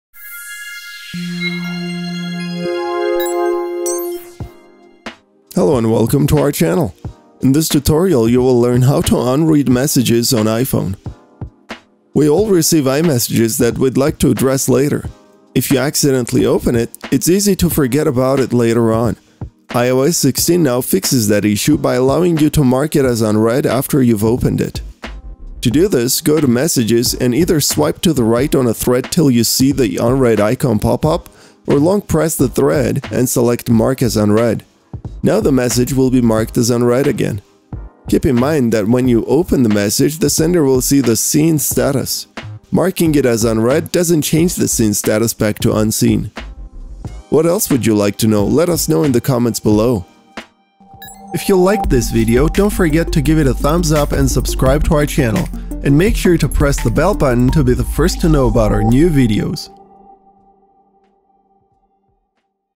iOS 16: How to Unread Messages on iPhone
In this tutorial, you will learn How to Unread Messages on iPhone.

We all receive iMessages that we’d like to address later. If you accidentally open it, it’s easy to forget about it later on. iOS 16 now fixes that issue by allowing you to mark it as unread after you’ve opened it.

To do this, go to Messages and either swipe to the right on a thread till you see the unread icon pop op or long-press the thread and select mark as unread. Now the message will be marked as unread again.

Keep in mind, that when you open the message, the sender will see the “seen” status. Marking it as unread, doesn’t change the “seen” status back to “unseen”.